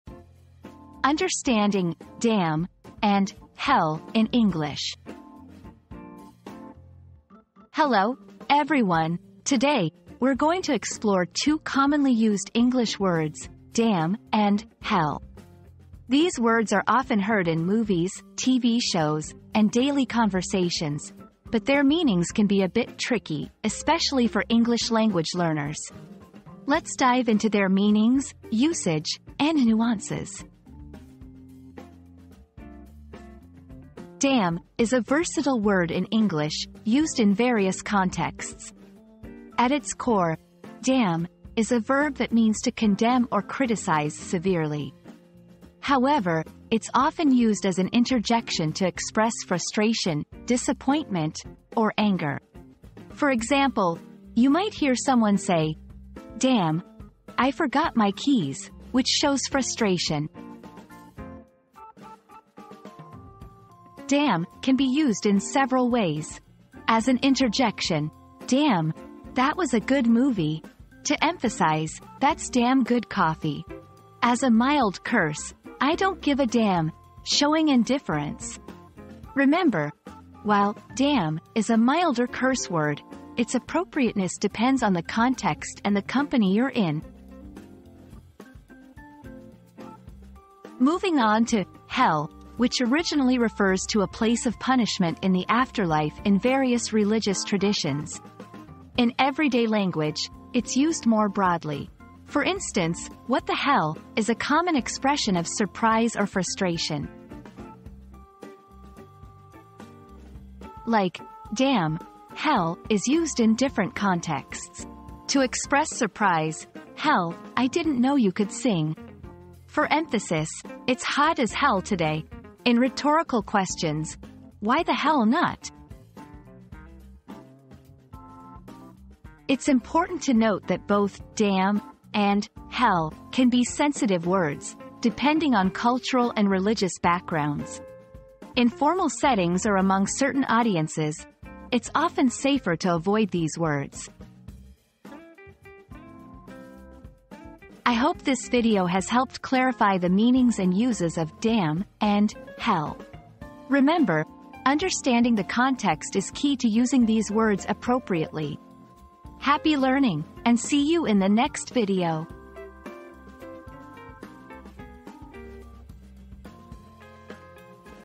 Understanding "Damn" and "Hell" in English
Unlocking the Secrets of English: Decoding 'Damn' and 'Hell' • Discover the true meanings and usage of 'Damn' and 'Hell' in English. From profanity to emphasis, this video explores the various contexts and nuances of these commonly used expressions. Expand your language skills and gain a deeper understanding of these intriguing words!

00:00 • Introduction - Understanding "Damn" and "Hell" in English
00:33 • The Meaning of "Damn"
01:03 • Different Uses of "Damn"
01:30 • The Meaning of "Hell"
01:50 • Different Uses of "Hell"
02:08 • Cultural Context and Sensitivity